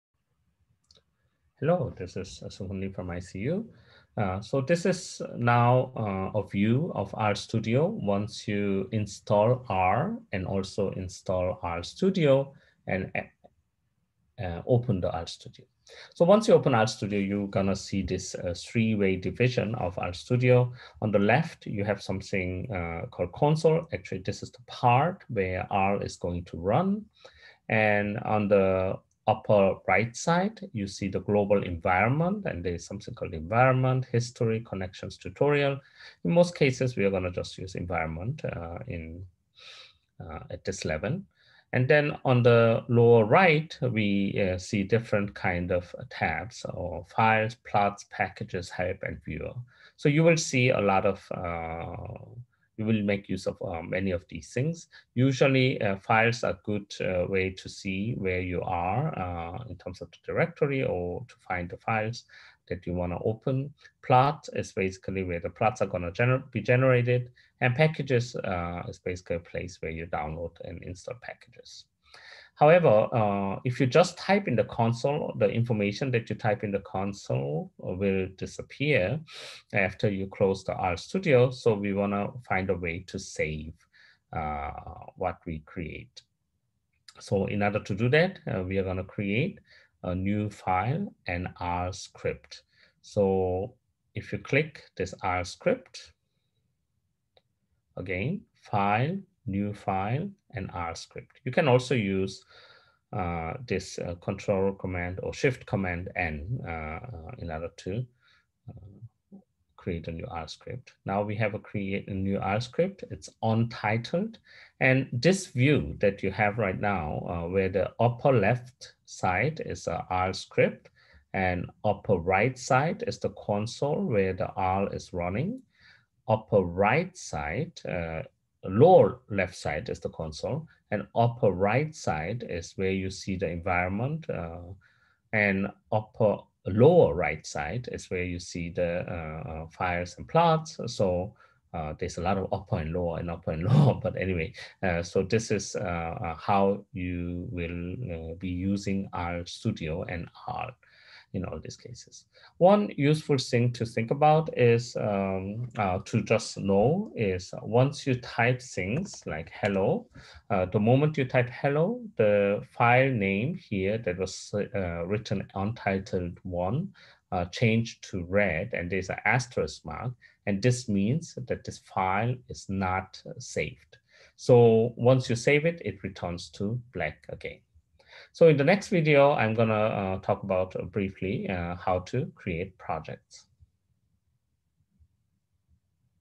[2-2 (E) R] R Studio: opening it for the first time
This video explains what you see when you first open R Studio.

International Christian University (Tokyo)
Linguistics Lab
Dr. Seunghun J. Lee